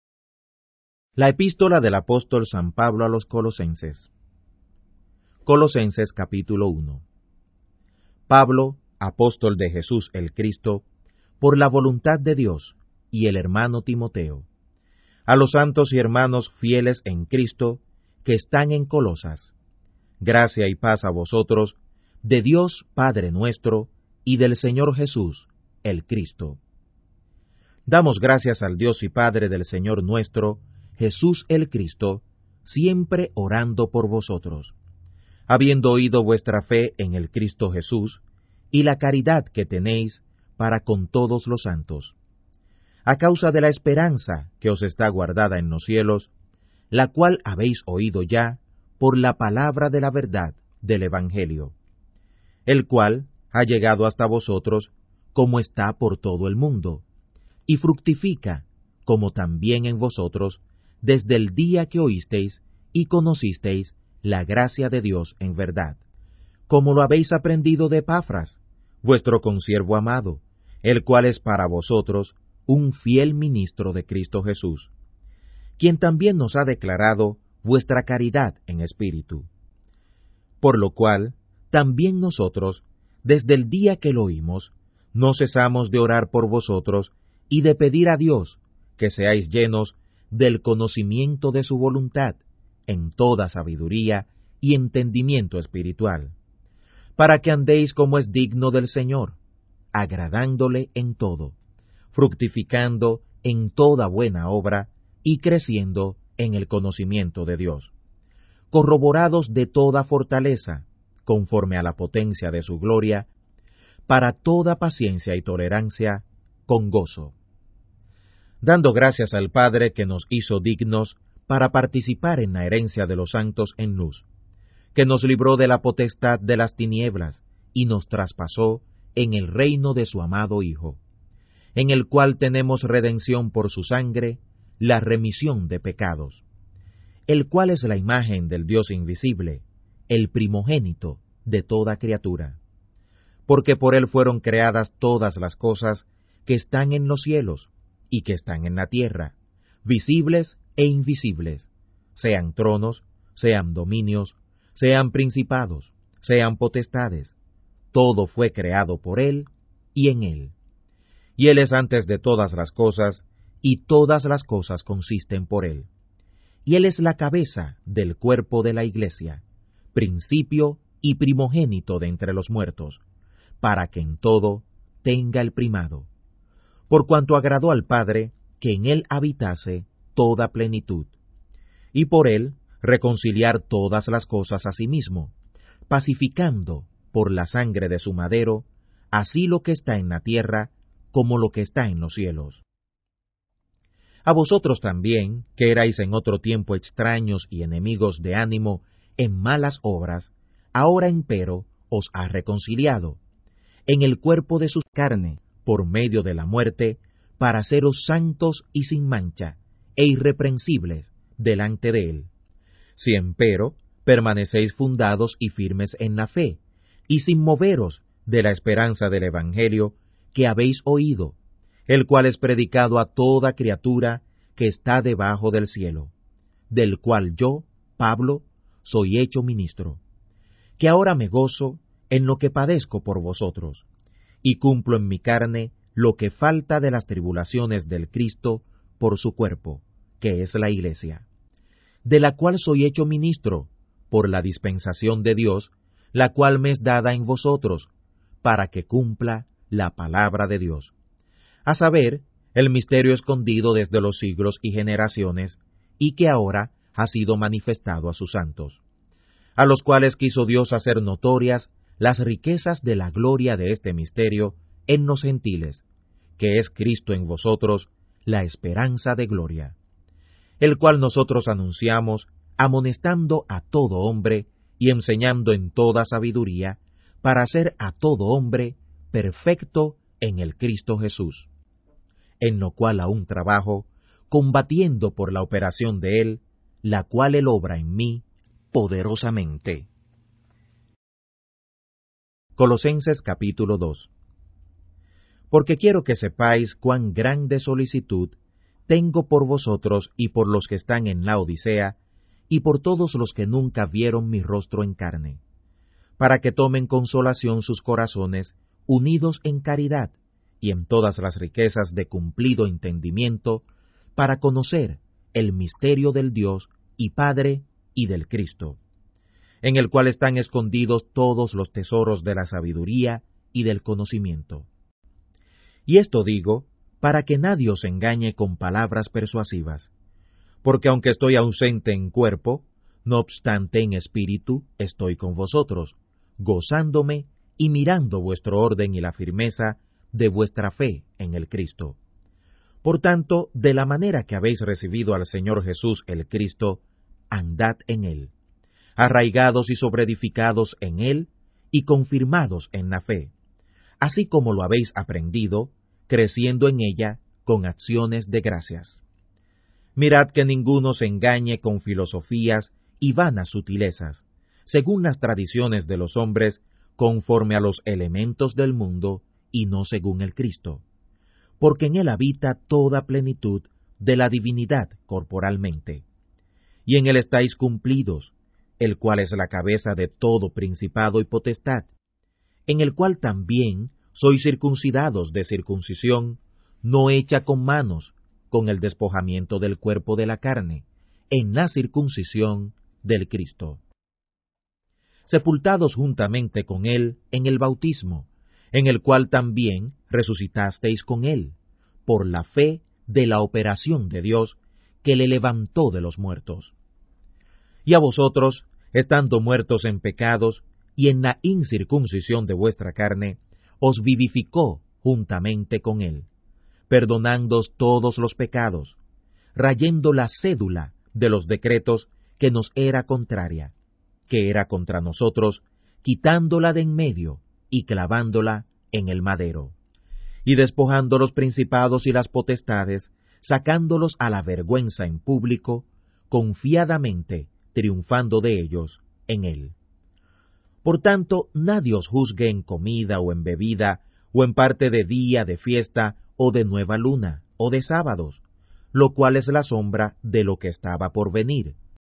Colosenes Parte 1
Word for Word Bible Drama from the Spanish Bible made with Christian Clip Art. Use these free Spanish Books of the Bible videos to learn and increase your faith.